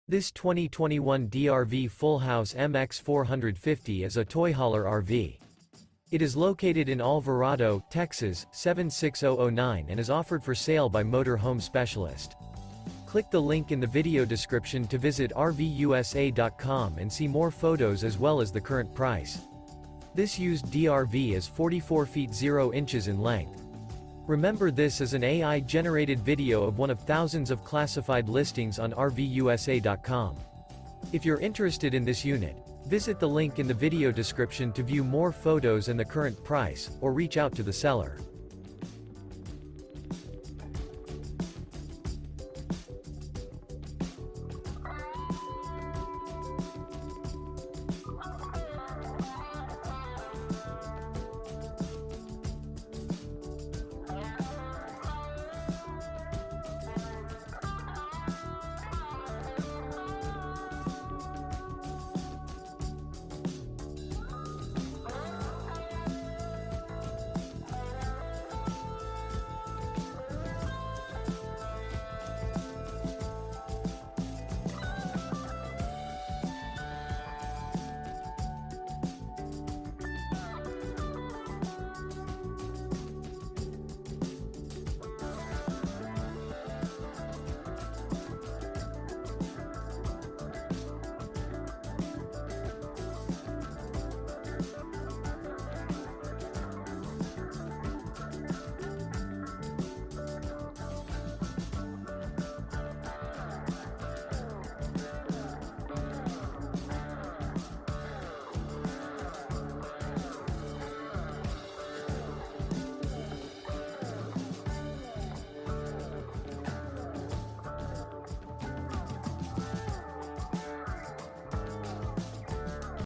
Eye-catching 2021 DRV Full House Toy Hauler RV For Sale in Alvarado, TX | RVUSA.com
This 2021 DRV Full House MX450 is a Toy Hauler RV. It is located in Alvarado, TX 76009 and is offered for sale by Motor Home Specialist. This Used DRV Is 44' 0" in length.